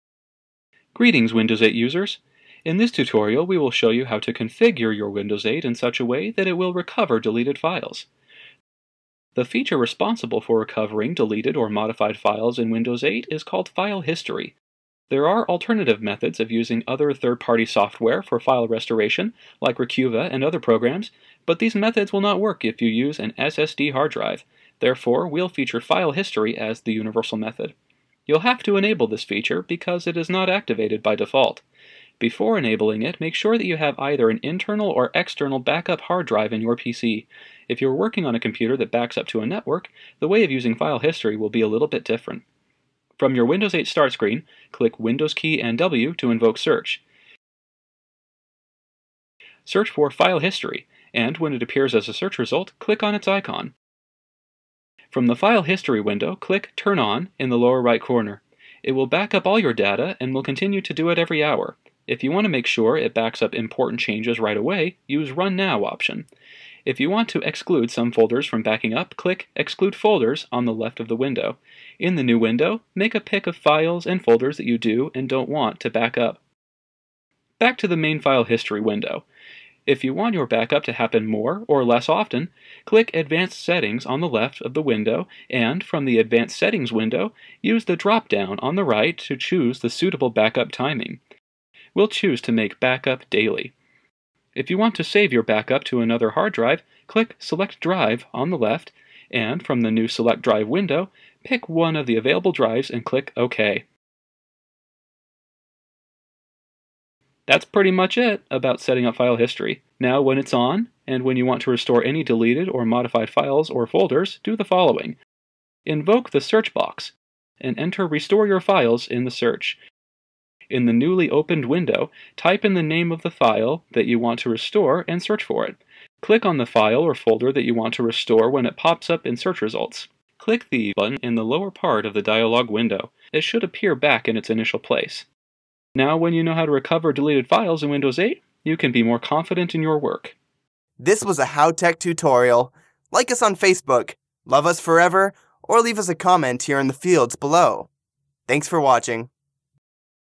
How to Recover Deleted Files with File History in Windows 8
Greetings, Windows 8 users! In this tutorial we will show you how to configure your Windows 8 in such a way that it will recover your deleted files. We will demonstrate it using Windows 8.1 version.

The feature responsible for recovering deleted or modified files in Windows 8 is called "File History". There are alternative methods of using other third-party software for file restoration (like Recuva and other programs), but these methods will not work if you use an SSD hard drive. Therefore we'll feature File History as the universal method.

You'll have to enable this feature, because it is not activated by default.  Before enabling it, make sure that you have either an internal or external backup hard drive in your PC. If you're working on a computer that backs up to a network, the way of using File History will be a little bit different.

Step 1.  From your Windows 8 start screen, click "Windows Key + W" to invoke "Search". Search for "File History" and, when it appears as a search result, click on its icon.

Step 2. From the File History window, click "Turn on" in the lower right corner. It will back up all your data and will continue to do it every hour. If you want to make sure it backs up important changes right away, use "Run now" option.

Step 3.  If you want to exclude some folders from backing up, click "Exclude folders" on the left of the window. In the new window, make a pick of files and folders that you do and don't want to back up.

Step 4. Back to the main File History window. If you want your backup to happen more or less often, click "Advanced settings" on the left of the window and, from the Advanced Settings window, use the drop-down on the right to choose the suitable backup timing. We'll choose to make backup daily and save changes.

Step 5.  If you want to save your backup to another hard drive, click "Select drive" on the left and, from the new Select Drive window, pick one of the available drives and click "OK".

That's pretty much it about setting up File History.

Step 6.  Now when it's on and when you want to restore any deleted or modified files or folders, do the following. Invoke the "Search" box and enter "restore your files" in the search.

Step 7. In the newly opened window, type in the name of the file or folder that you want to restore and search for it.

Step 8. Click on the file or folder that you want to restore when it pops up in search results. It should appear back in its initial place.

Result: Now when you know how to recover deleted files in Windows 8, you can be more confident in your work.